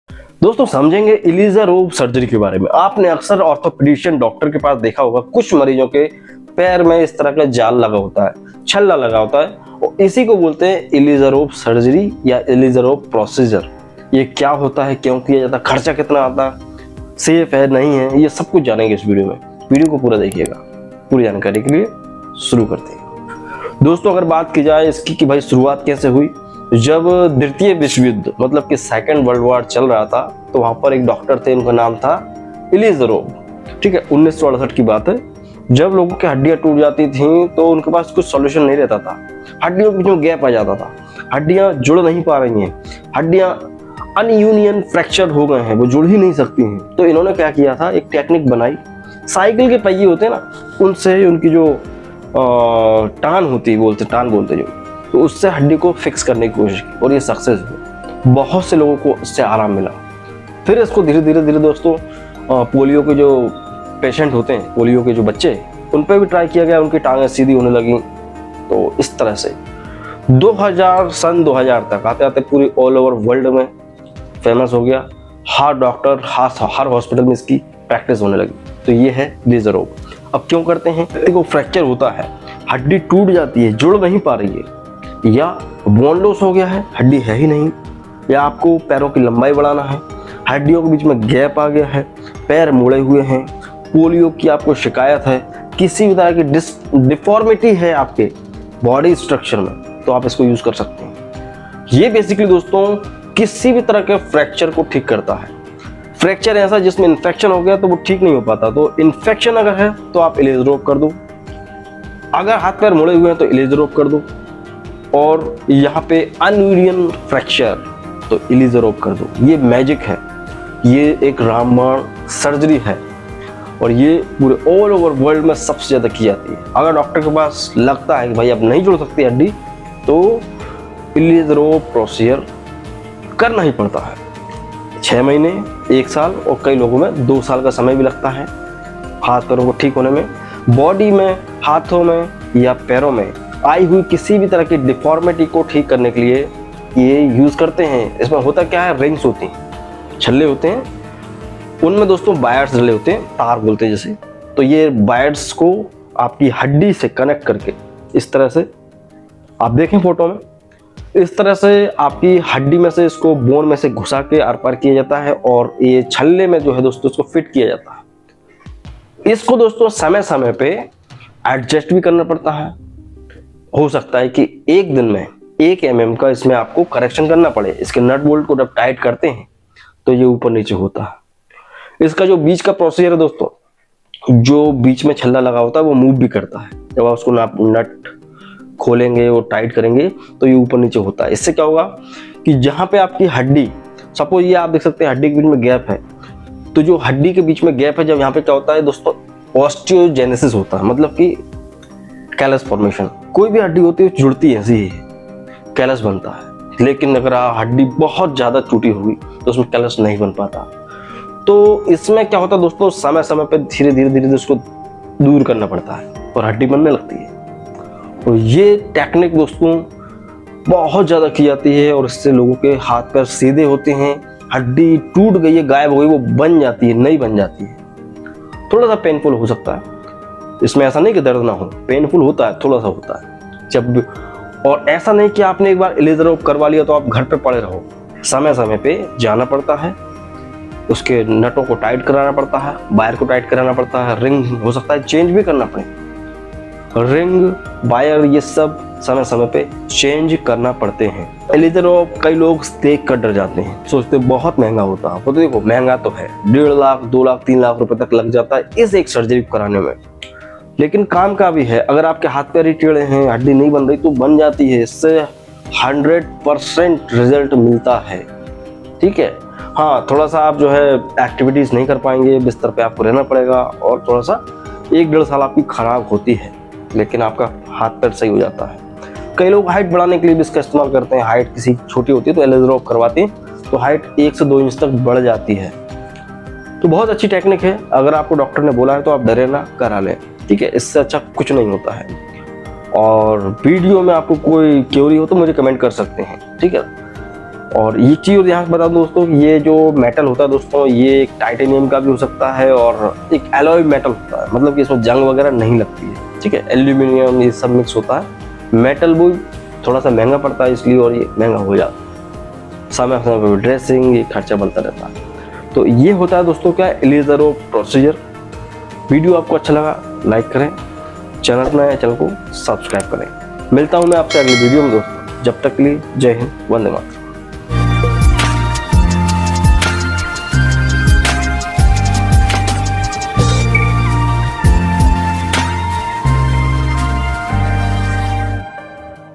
Ilizarov Surgery क्या है कैसे किया जाता है | Ilizarov Technique In Hindi 🔥 Orthopaedic Surgery
Hi Friends Welcome To Gyanear The Medical Channel I'm Shivam A Registered Pharmacist

Ilizarov treatment is a highly successful orthopedic surgical technique designed to lengthen or straighten bone and soft tissue.

The Video Content Is Not Intnded To Be A Substitute For Professional Medical Advice, Diagnosis Or Treatment

Thanks
Drx Shivam Vishwakarma